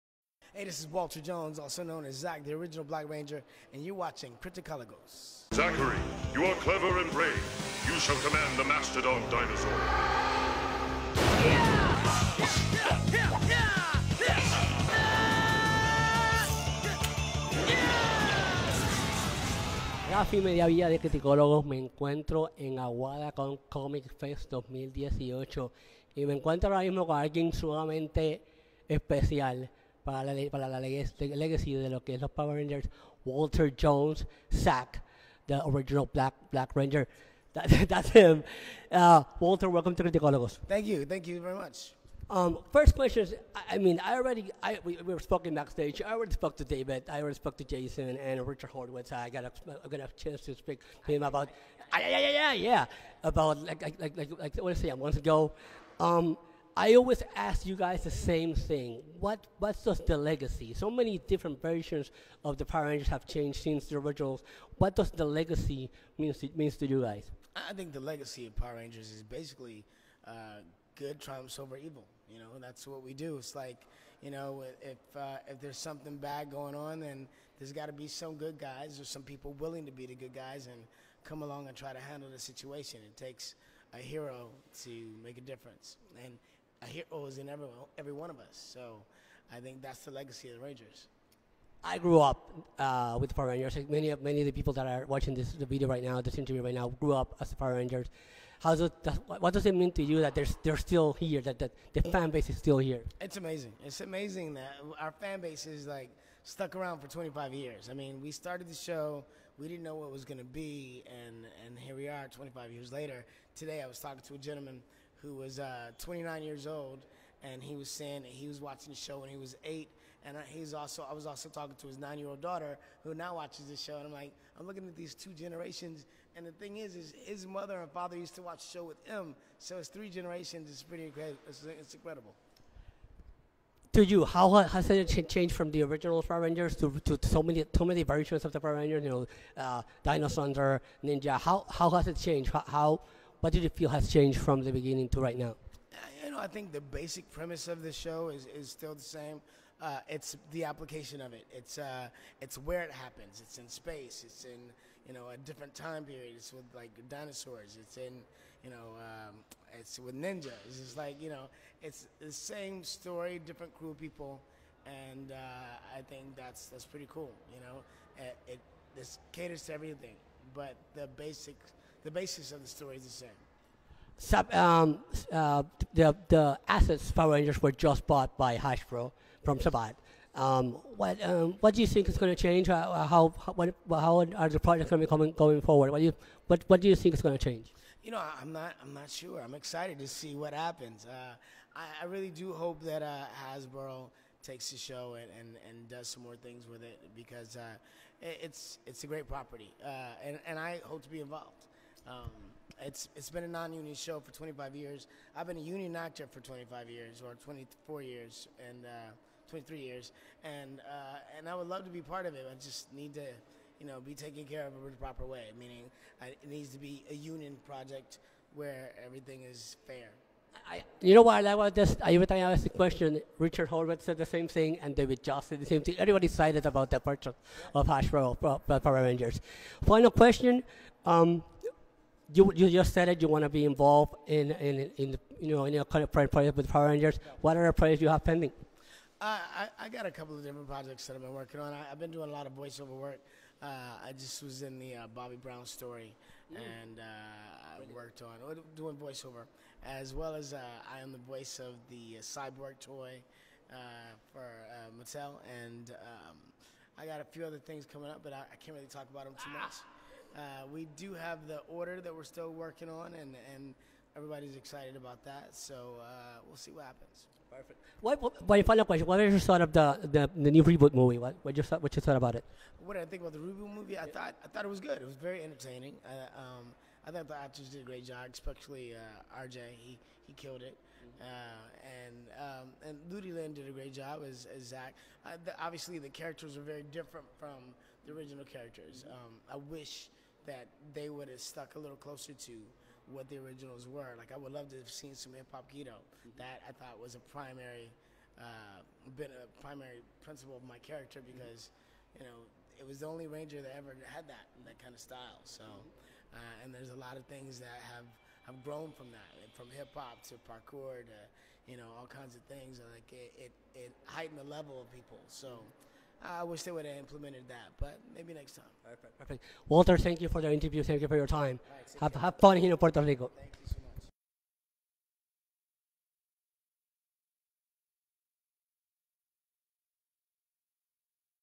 Interview Walter E. Jones / Comic Fest Aguada Con 2018
Nuestra entrevista (Interview) con el actor Walter Emanuel Jones Fan Page (@walterejones) a.k.a. Zack the original Black Ranger en Mighty Morphin Power Rangers, por el Criticologo, Rafy Mediavilla desde Comic Fest Aguada Con 2018. Enjoy!